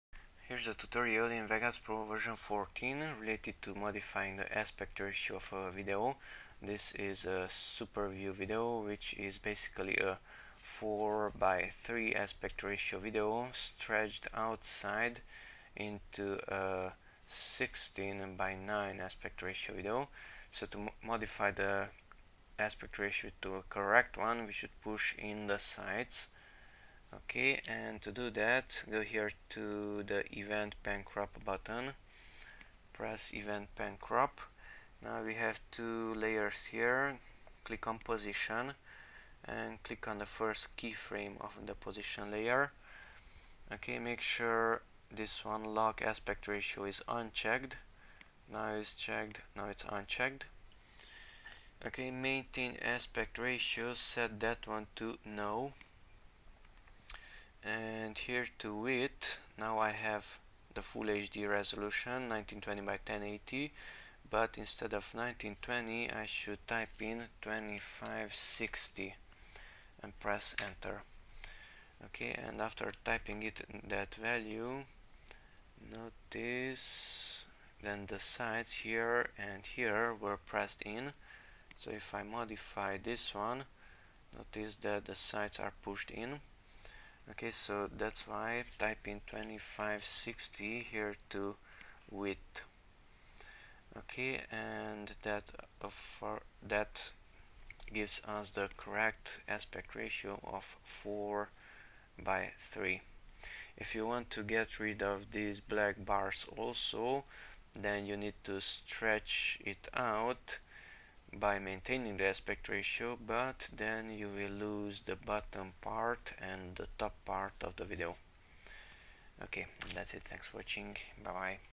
How to modify the Aspect Ratio of a video in Vegas Pro (SuperView, 16:9 to 4:3, No Crop)
How to push in the sides of a wide video that was captured in the 4:3 aspect ratio.
Instead of 1920 x 1080 use 2560 x 1080.